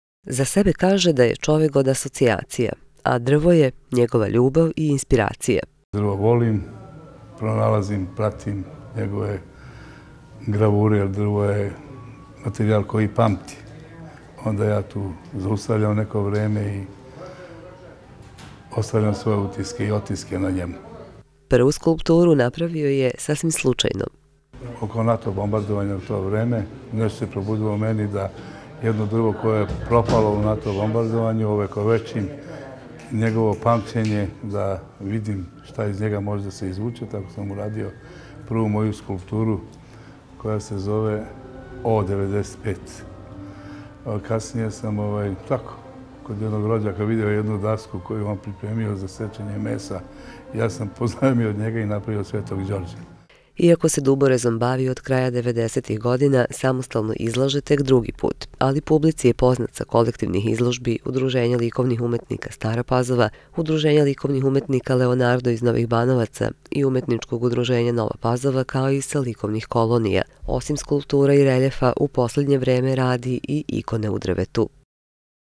Izlozba Dragomir Rudovic
U galeriji CzK otvorena je izložba reljefa i skulptura u drvetu Dragomira Bate Rudovića.